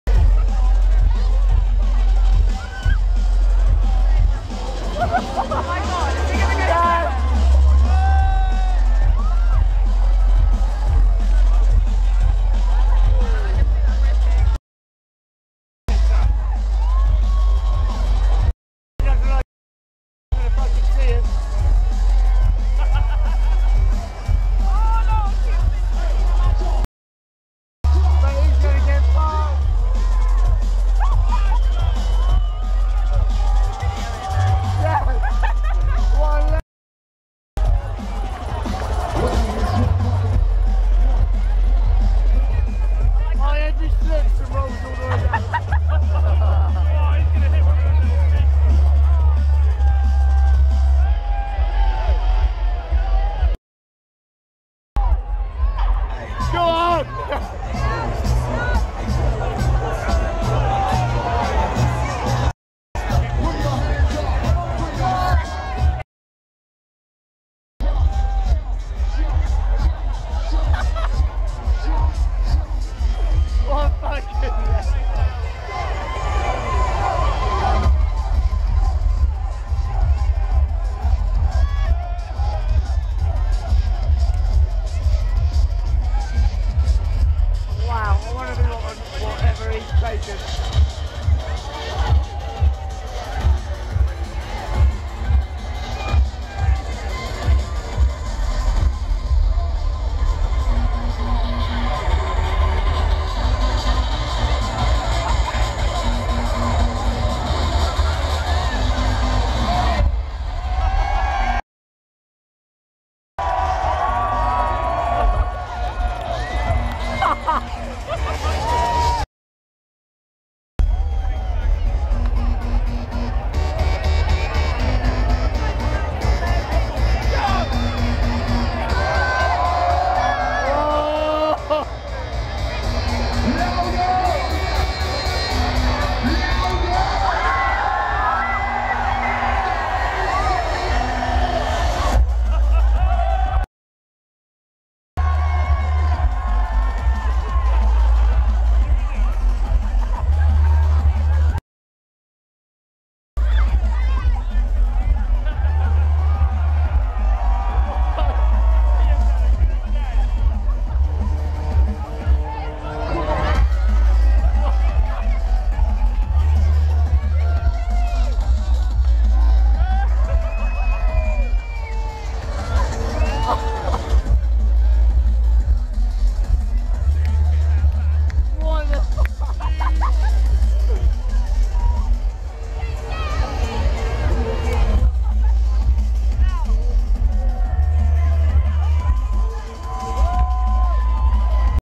Deadmau5 at LED London. Guy climbs marquee. Epic Faceplant.
LED Festival, London 2011. During Nero's epic performance in the Lets Go Crazy Arena, this guy was clearly too excited about seeing deadmau5, so much so he quite obviously got kicked out for something I have never seen at a festival! If you don't want to watch it all, make sure you watch the end for the most epic rope face plant I have ever seen. I also hope by uploading this video, that the legend who stars can be identified and given many deserved high 5's... So if you have seen this, please like it!